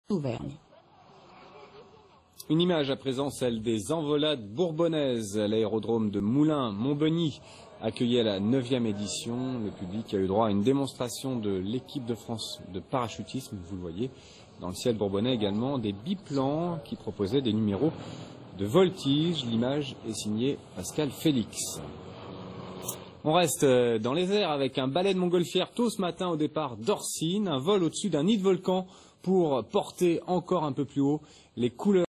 France 3 Auvergne - 27 septembre 2015 - 8e édition des Envolades Bourbonnaises
Retrouvez le reportage consacré à la 8e édition des Envolades Bourbonnaises.